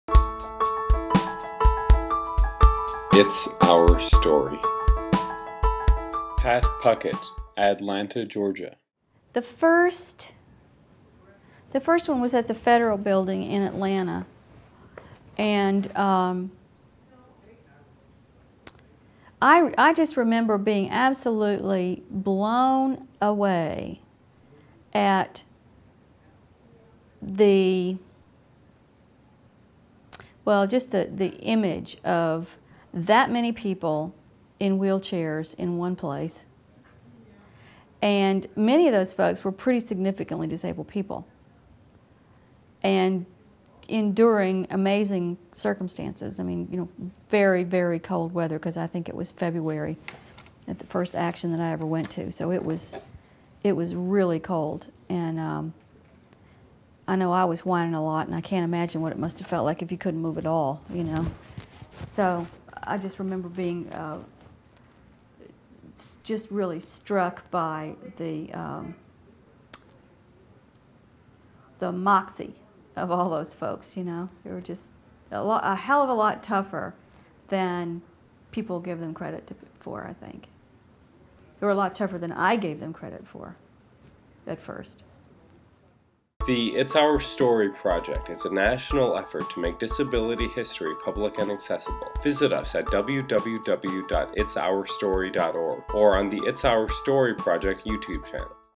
Pat Puckett, part 08 of 14: "Protest in Atlanta"
Pat Puckett of Atlanta, GA speaks about the systemic problems and causes of the institutionalization of people with disabilities.

Pat acquired her physical disability when she was infected with polio at an early age; she has advocated for disability rights throughout her life, including engaging in civil disobedience with ADAPT.  She currently serves as executive director of the Statewide Independent Living Council of Georgia.

This interview transcribed by Guadalupe Samuels.